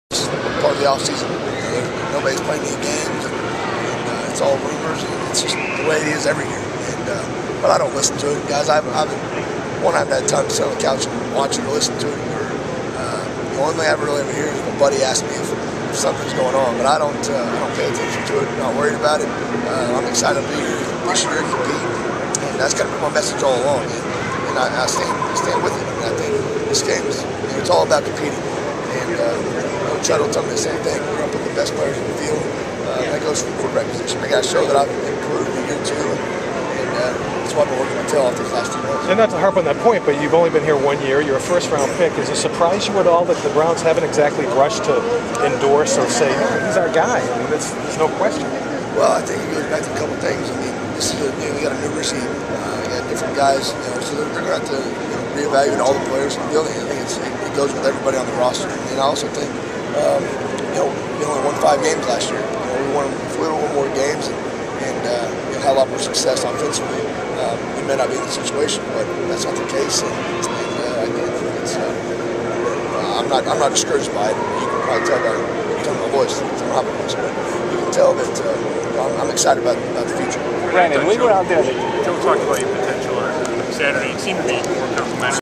Brandon Weeden talks about competing for his job
Brandon Weeden at the IX Center